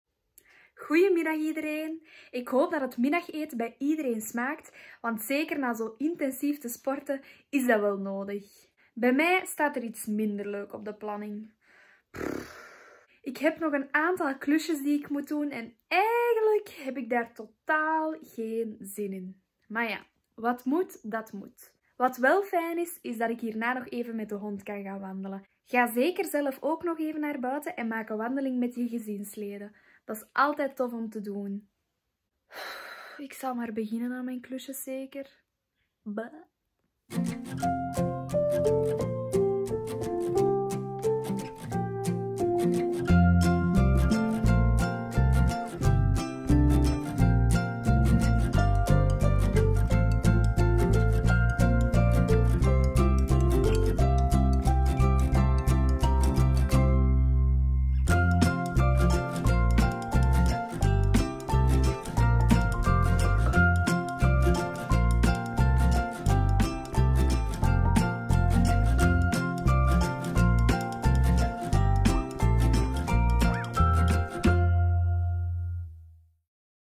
Middagpauze - Donderdag - 12u00
✌️MIDDAGPAUZE ✌️

Geniet van je lunch!

Ondertussen kunnen jullie de benen strekken en buiten even een luchtje gaan scheppen.

Om 13u zijn we er terug met een leuke creatieve opdracht.

Smakelijk!

Volg ons voor meer leuke video's!